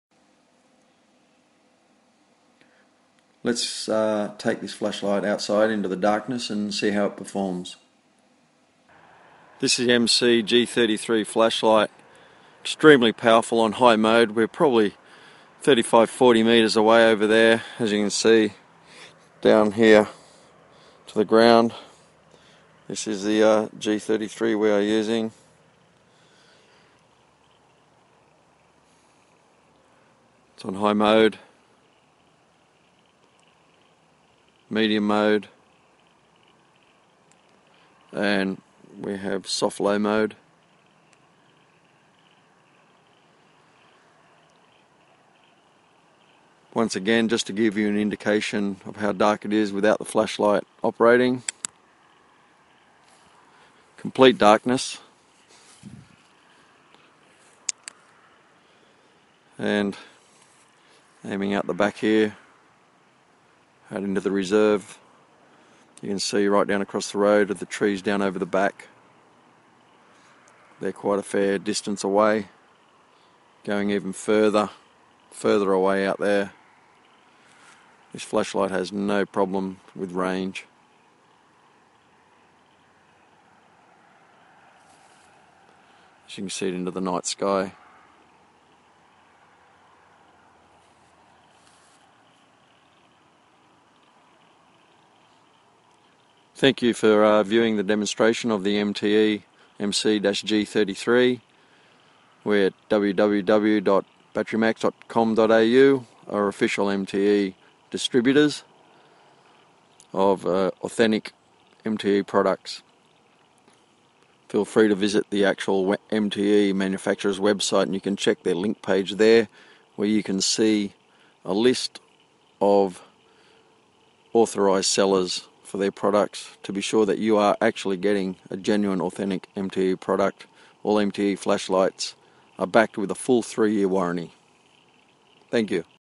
MTE 1000 lumen Model MC-G33 Flashlight Demonstration Test
The MTE MC-G33 Cree XM-L flashlight is one of the most powerful
lumens to size ratio flashlight operating on a single 18650 battery.
The most outstanding feature of this flashlight is how the new XM-L
emitter combines with its near flawlessly designed reflector to provide a
tight beam pattern outperforming most anything in its class.